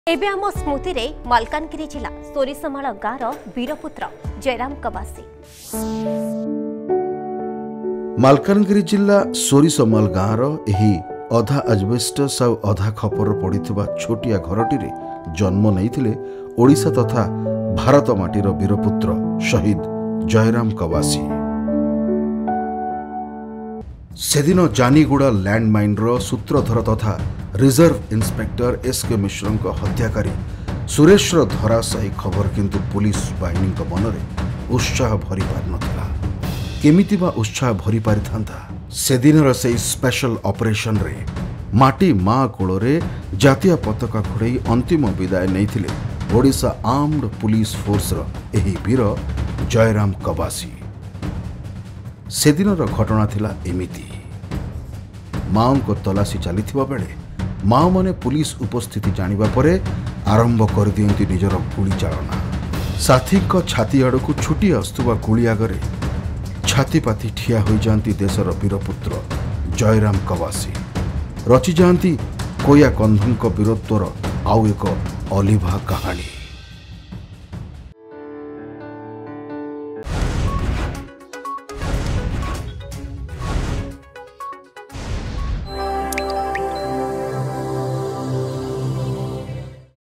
ଗରିବଙ୍କ ଗାଁ ଆଜି ସହିଦଙ୍କ ସ୍ମୃତିରେ ଧନୀ ।
ସହିଦ ଜୟରାମ ଦେଲେ ନିଜ ଗାଁକୁ ନୂତନ ପରିଚୟ ।